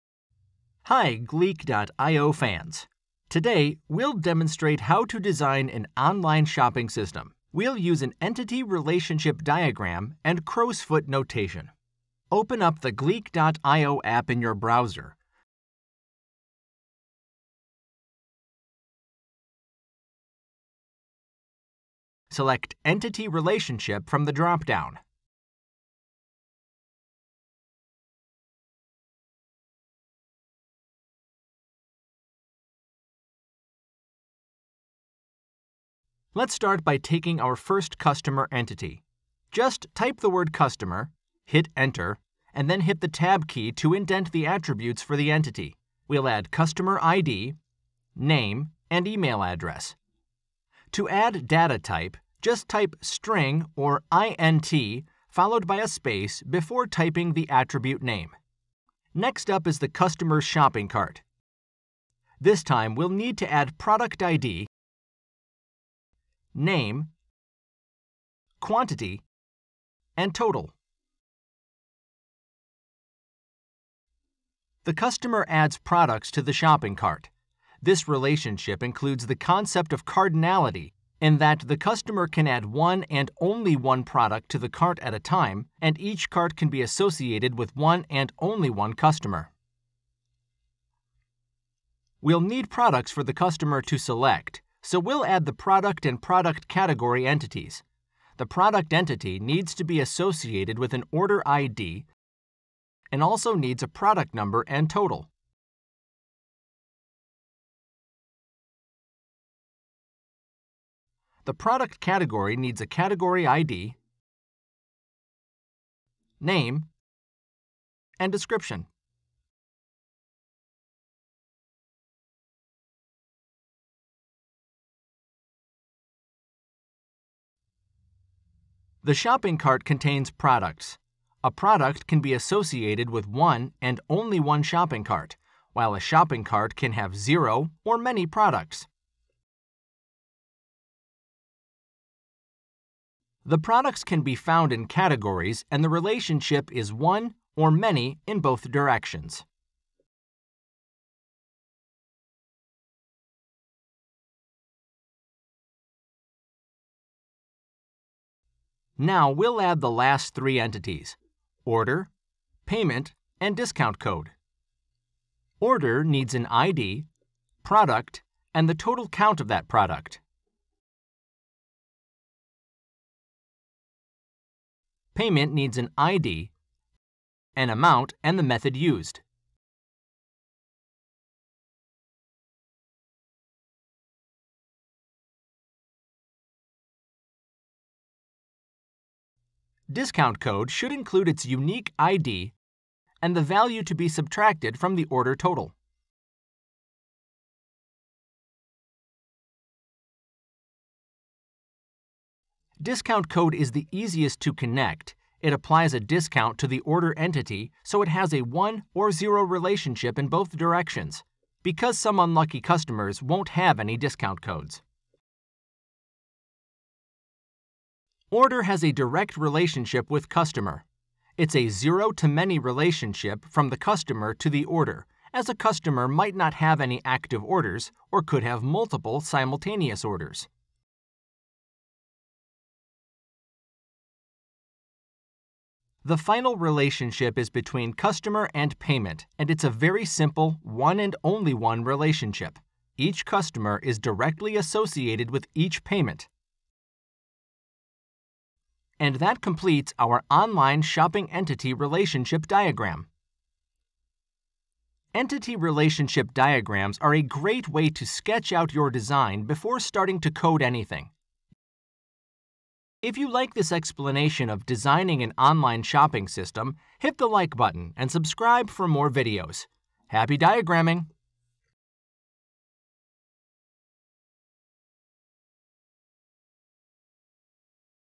ER diagram Crow's foot example: Online shopping system
Today we’ll demonstrate how to design an online shopping system. We’ll use an entity-relationship diagram and Crow's Foot notation.

Open up the Gleek.io app in your browser. Select entity-relationship from the dropdown.

Let’s start by creating our first customer entity. Just type the word Customer, hit Enter, and then hit the Tab key to indent the attributes for the entity. To add data type, just type string or int followed by a space before typing the attribute name. Use string if the attribute is composed of letters and numbers, use int if it’s just numbers.

Next up is the customer’s shopping cart. This time we’ll need to add product ID, name, quantity, and total.

The customer adds products to the shopping cart. This relationship includes the concept of cardinality, in that the customer can add one and only product to the cart at a time and each cart can be associated with one and only one customer.

We’ll need products for the customer to select, so we’ll add the product and product category entities. The product entity needs to be associated with an order ID and also needs a product number and total. The product category needs a category ID, name, and description.

The shopping cart contains products. A product can be associated with one and only one shopping cart, while a shopping cart can have zero or many products. The products can be found in categories and the relationship is one or many in both directions.

Order has a direct relationship with customer. It’s a zero to many relationship from the customer to the order, as a customer might not have any active orders or could have multiple simultaneous orders.

The final relationship is between customer and payment and it’s a very simple one and only one relationship. Each customer is directly associated with each payment.

And that completes our online shopping entity-relationship diagram. Entity relationship diagrams are a great way to sketch out your design before starting to code anything.

Happy diagramming!